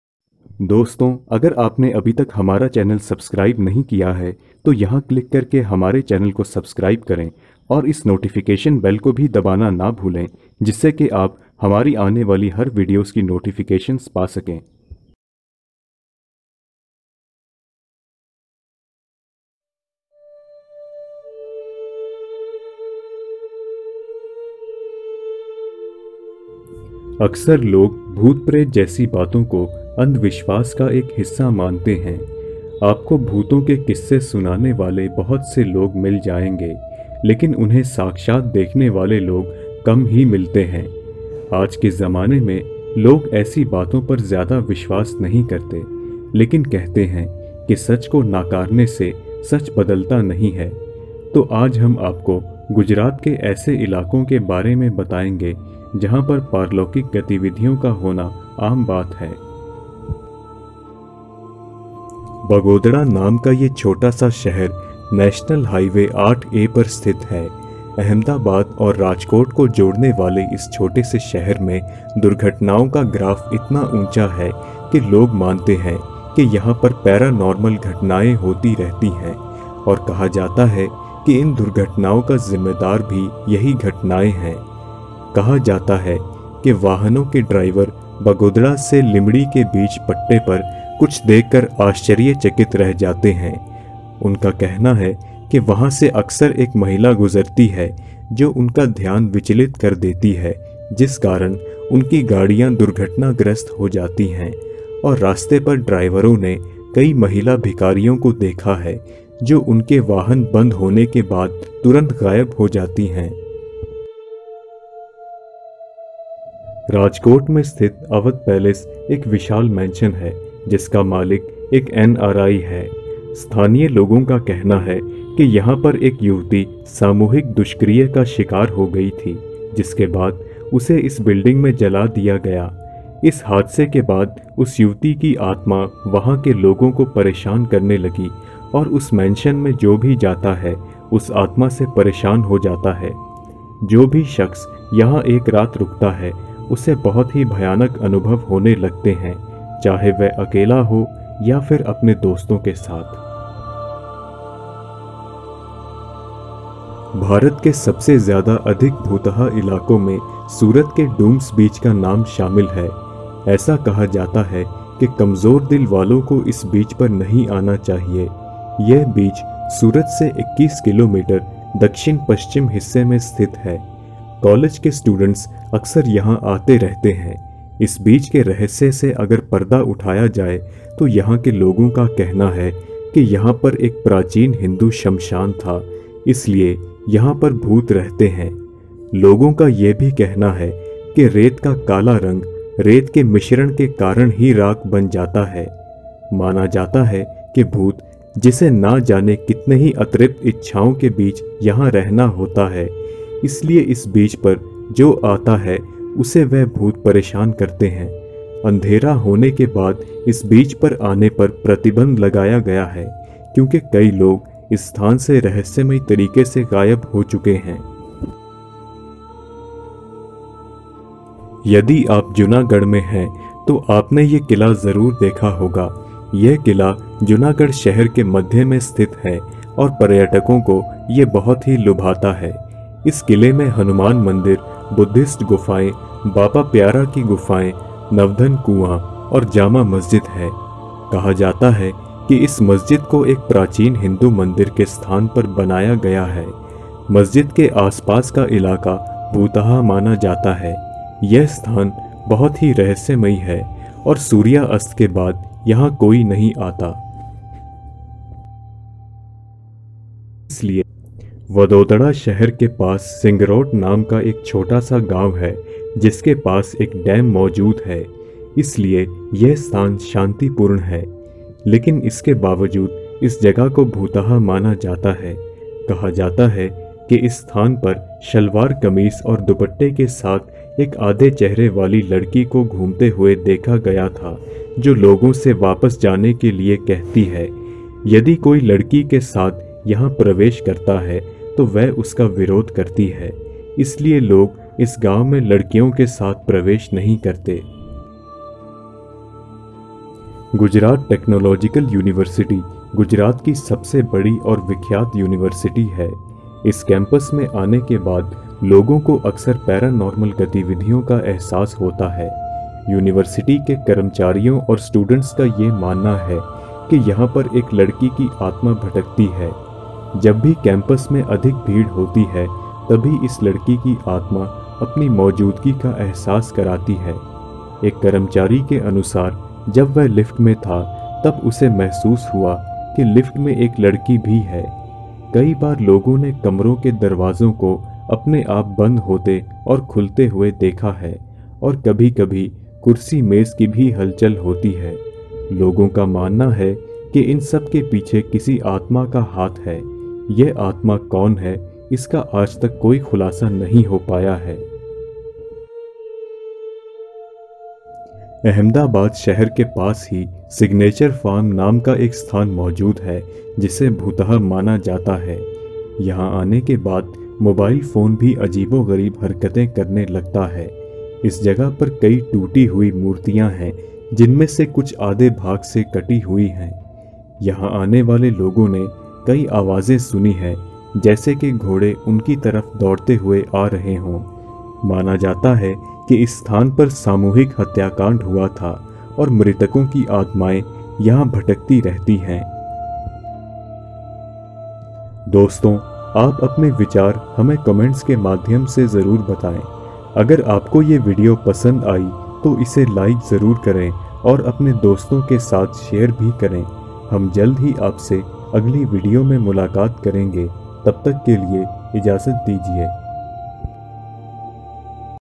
Gujarat top haunted places in hindi || Mysterious Nights India || Episode 35#
Hello guys, in this video, we will talk about some of the haunted places of Gujarat, namely –

1.Ahmedabad – Rajkot highway,
2.Awadh palace, Rajkot,
3.Dumas beach, Surat,
4.Uparkot fort, Junagarh,
5.Sindhrot, Vadodara,
6.GTU Campus, Ahmedabad.
7.Signature Farm, Ahmedabad.

To know more about these haunted places, please watch this video till end.